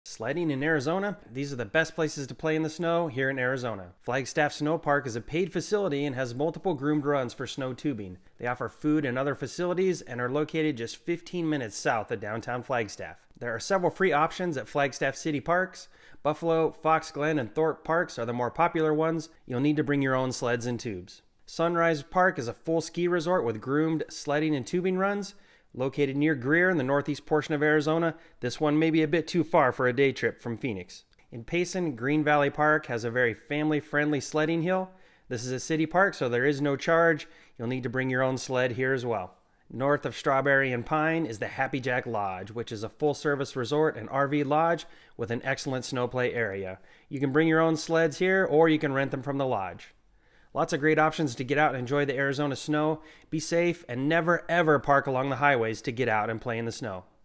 Where to go snow sledding in Arizona.
When most people think of winter in Arizona, images of cactus and warm desert scenes come to mind.

The cool thing about Arizona is the diversity of the landscape. Just north of Phoenix, the elevation increases and the countryside is filled with pine tree forests. These areas also experience all four seasons, including winter full of snow.

Quite a bit of snow actually. The elevation in Flagstaff is almost 7,000 feet and they regularly receive snow storms dropping over a foot of snow.

This creates a great opportunity for residents of Phoenix to jump in the car and make the two hour trip north and play in the snow.

Read the full blog post & get directions to all of the best spots

Have questions about life in Phoenix or need help with real estate?

J.D. Manning
eXp Realty